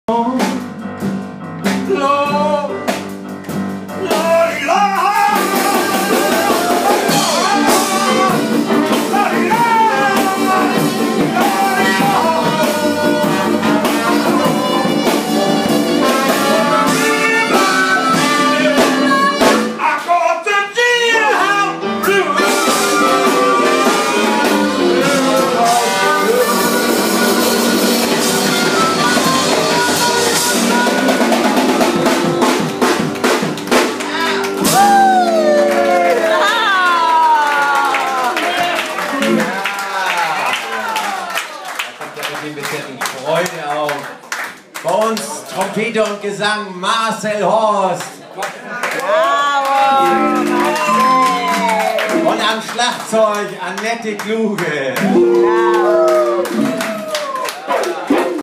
El cantante de Jazz en el Cotton Club de Hamburgo
En este Club ubicado en el Centro de Hamburgo se reúnen todos los primeros miércoles de todos los meses durante años este grandioso grupo de músicos profesionales, y la entrada es 7 Euros y tienes muchos Brezel recién salidos del horno una delicia, el mejor que he comido en todo Hamburgo, Vinos italianos, Pizza, una delicia con esta bella música de Jazz